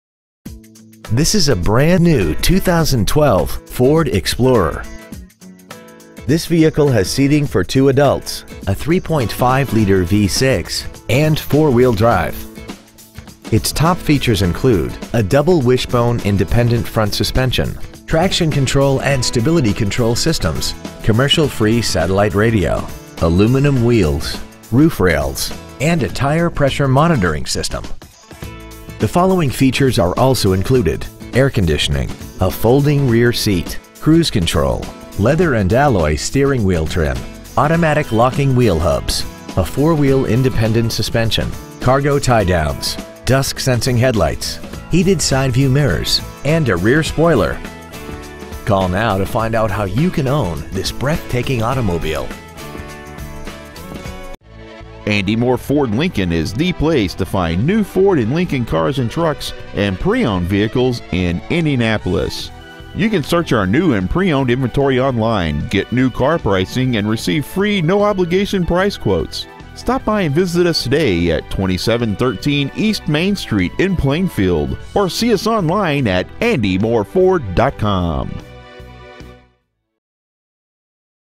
2012 Ford Explorer Plainfield IN 46168
Year : 2012
 Make : Ford
 Model : Explorer
 Engine : 3.5L V6 24V MPFI DOHC
 Trans . : 6-Speed Automatic
 Exterior : Dark Blue Pearl Metallic

 Interior : v
 Stock : T13769

 2713 E. Main Street